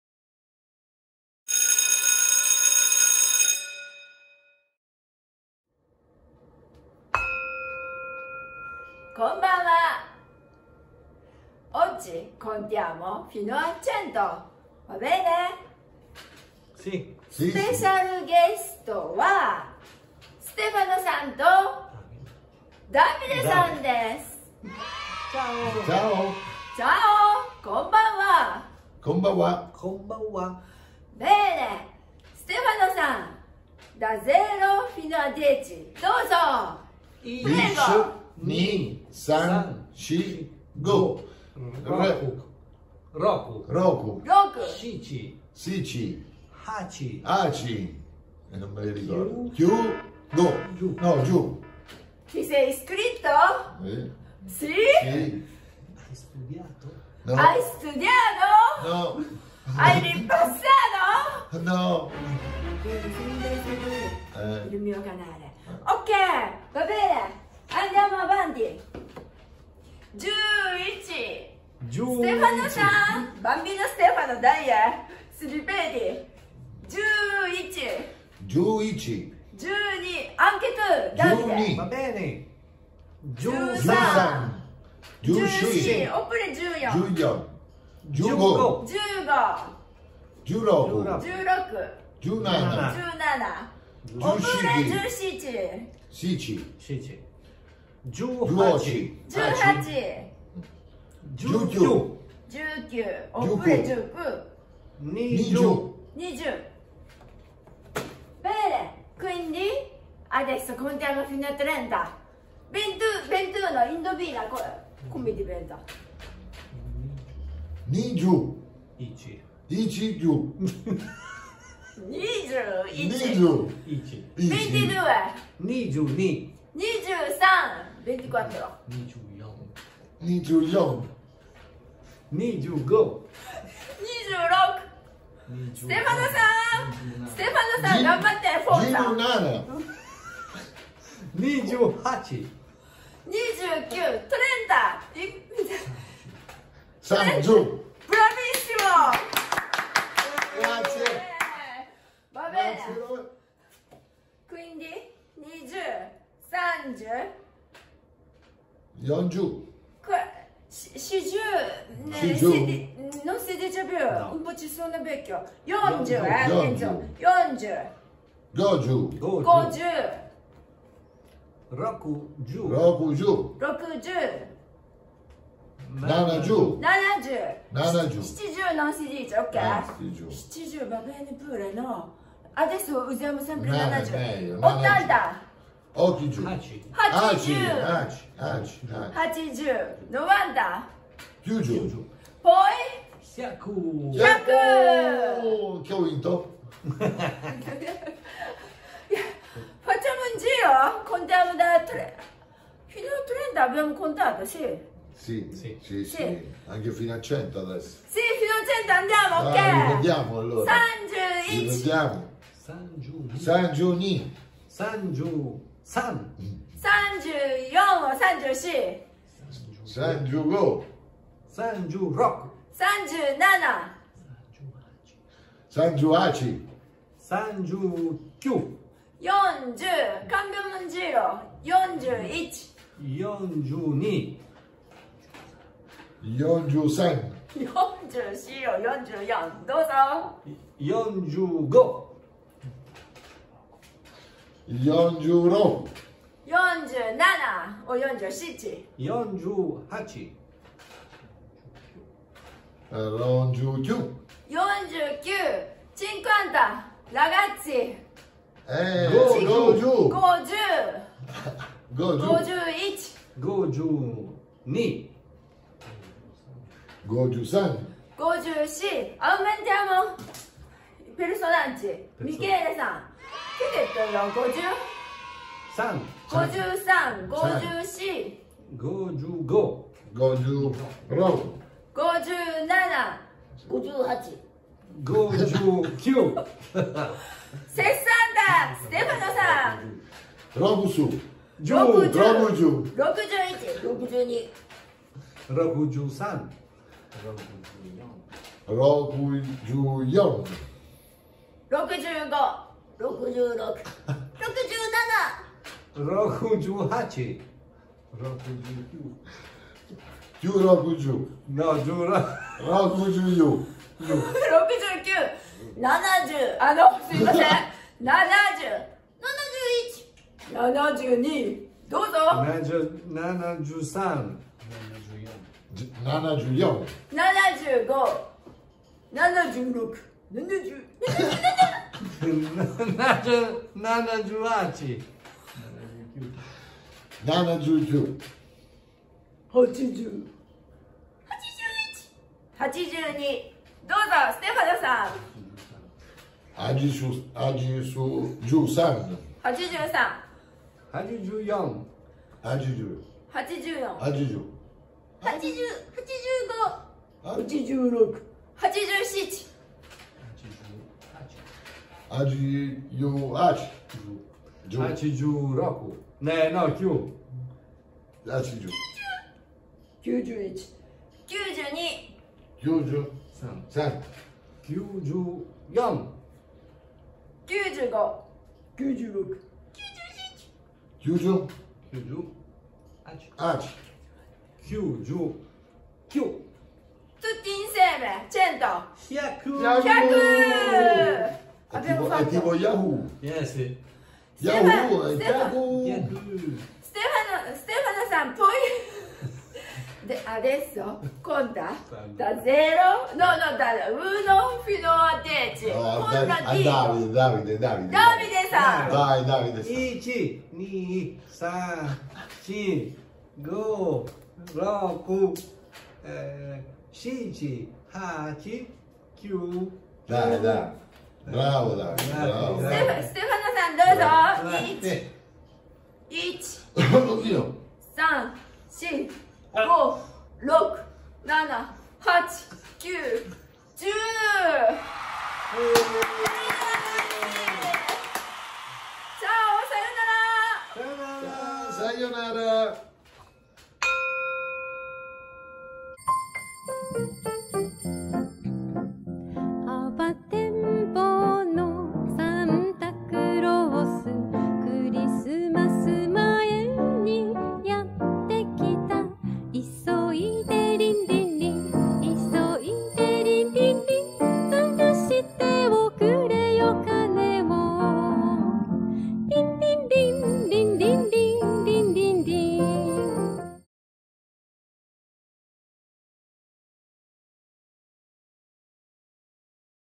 Lezione 7 Contiamo fino a 100
*numero 4 si pronuncia come morto ma si scrive diversamente.
Numero quattro è 四 mentre morto è 死 in ideogramma kanji.
Per questo motivo molta gente non ama il numero 4

Come tante altre parole una singola parola può avere molti significati e questo dipende dal contenuto della conversazione. Può cambiare anche in base ai kanji usati.

Cantante:Misa Sou宗美佐